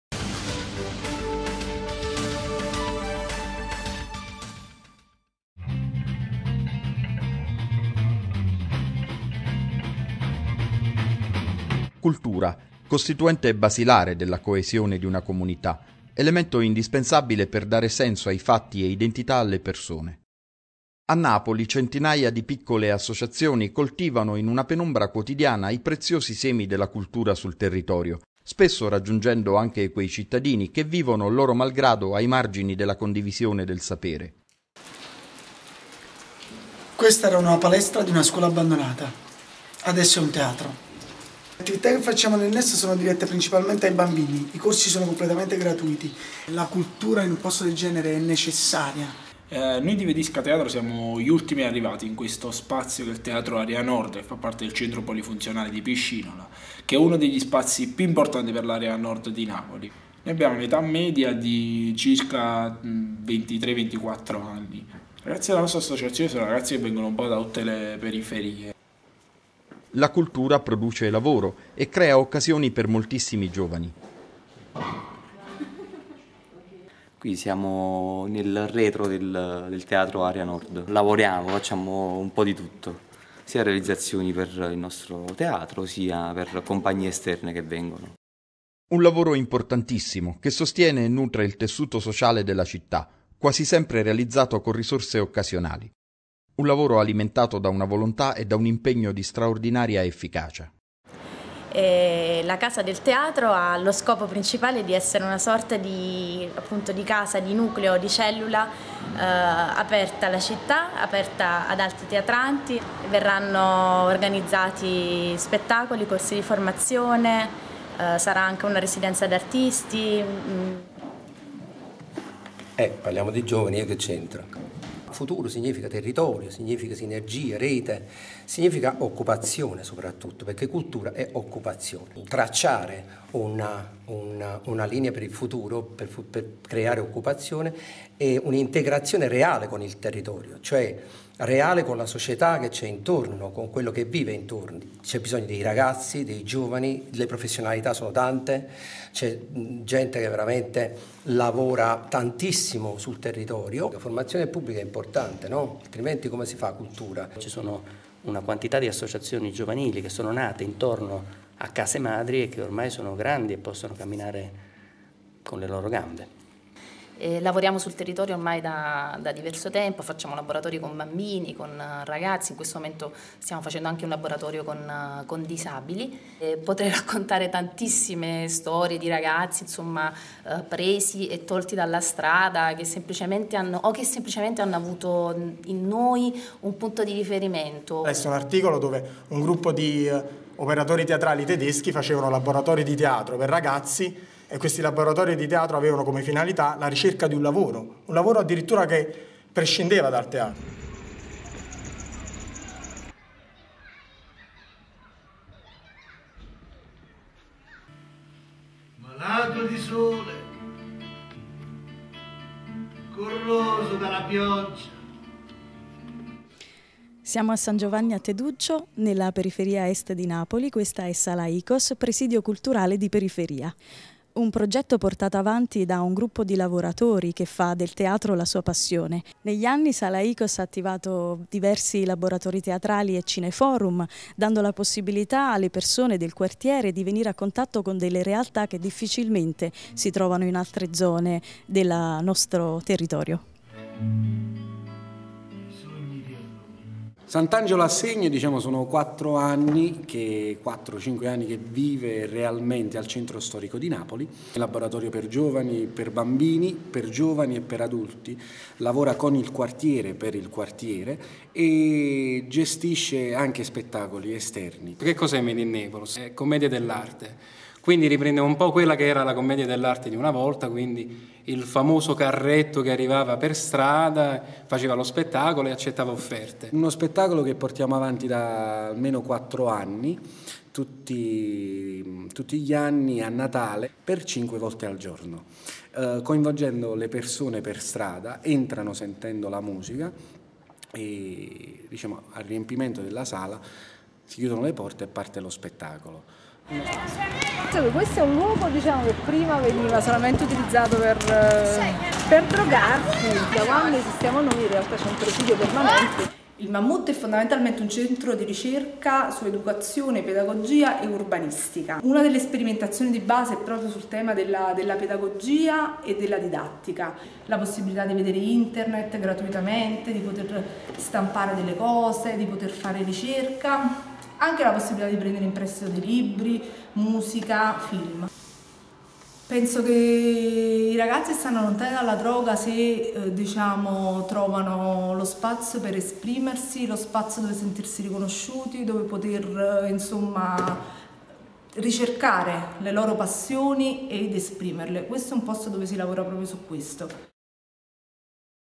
Piccole associazioni crescono
Uno sguardo sulle giovani realtà associative napoletane che producono lavoro, cultura, tessuto sociale.